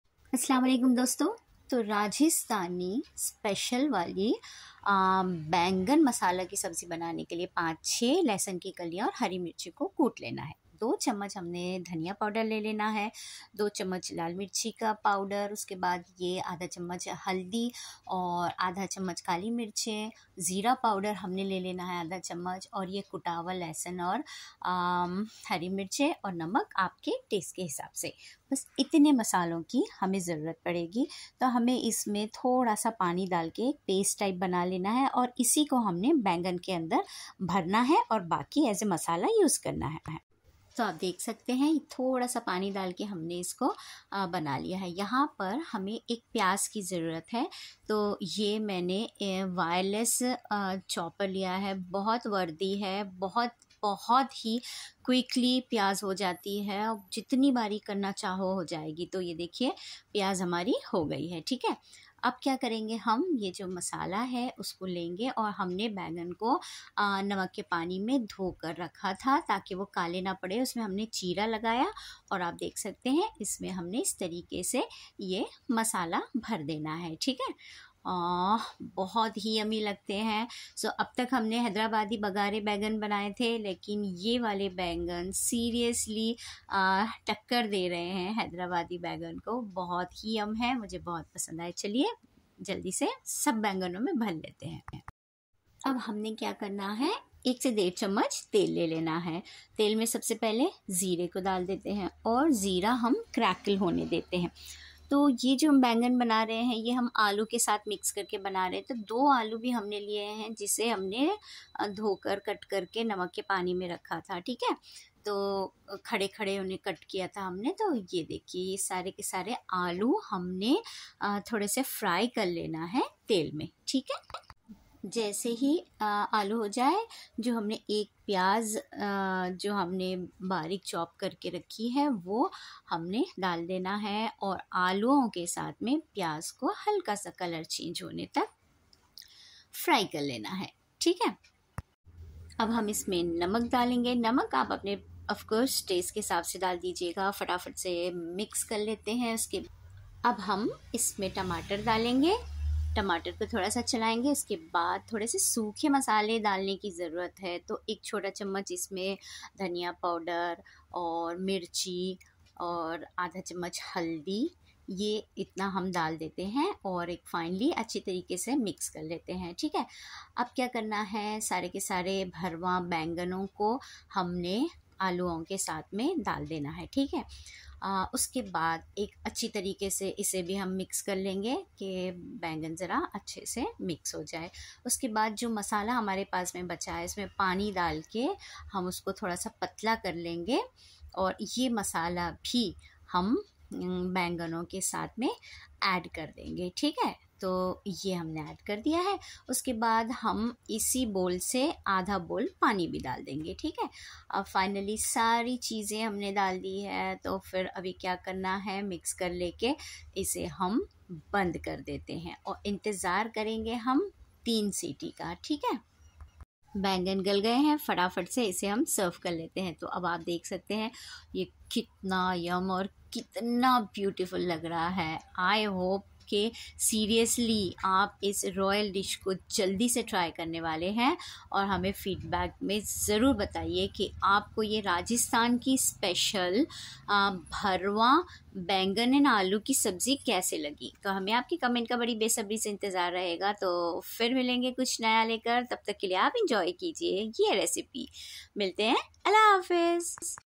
Rajasthani Bharwa Bengan || Royal recipe of Rajasthan|| राजस्थानी भरवा बैंगन|| Eggplant recipe
Aaj ham Rajasthan ki ek royal recipe aap se share kar rahe hain... bahut yummy banne wale bharva baingan ki gravy... baigan Jise pasand nahin hai vah agar ise ek bar khaenge to iske fan ban jayenge... ise zarur try karke Apne friends and family mein share karna na bhule...